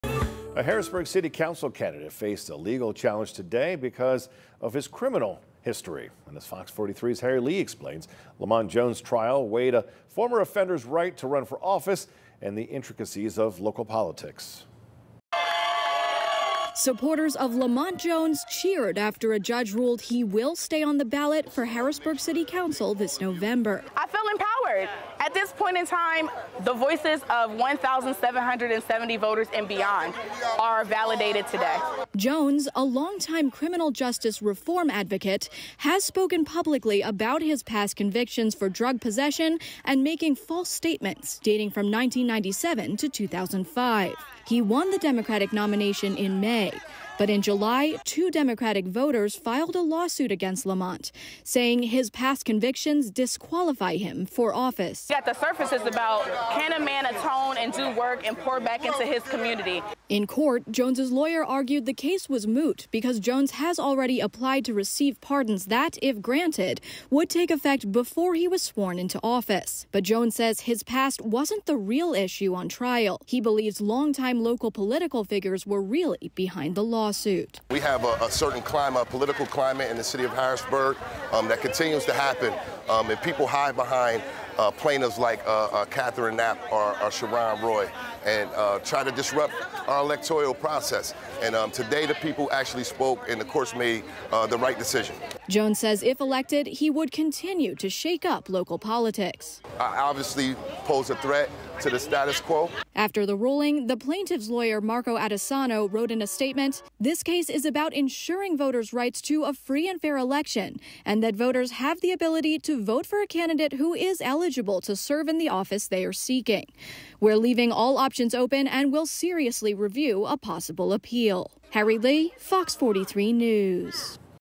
Judge rules that Harrisburg City Council candidate will remain on ballot
Harrisburg City Council candidate Lamont Jones will remain on the November ballot after a judge ruled his past criminal record did not disqualify him from office.

Jones has spoken publicly about his past convictions for making false statements, a misdemeanor, in 1997 and for felony drug possession with intent to distribute in 2004 and 2005.

Since then, Jones has become a criminal justice reform advocate. He previously led Harrisburg nonprofit Breaking the Chainz serves and currently runs his own company helping formerly incarcerated people reenter society. He serves on several public advisory boards that deal with criminal justice issues, such as the Dauphin County Reentry Coalition.

The Pennsylvania state constitution bans those who have been convicted of “infamous crimes” from holding office in the Commonwealth.